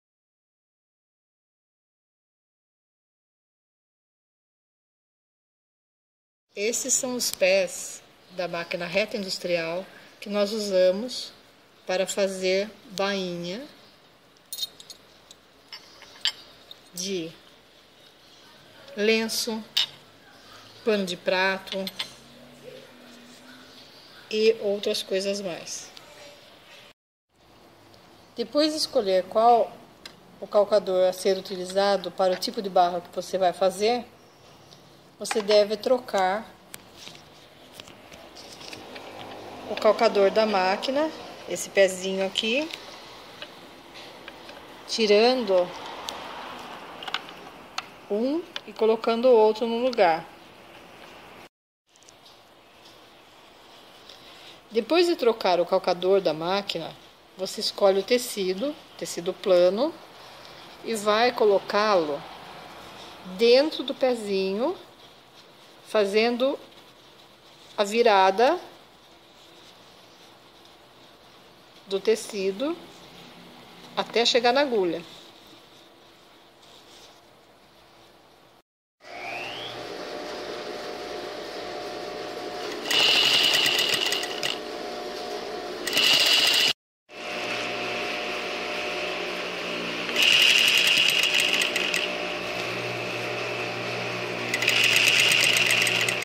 Como utilizar calcador/pé de embanhar/fazer barra na máquina reta industrial
Aprenda a utilizar os calcadores de embanhar ou pé de fazer bainha na máquina.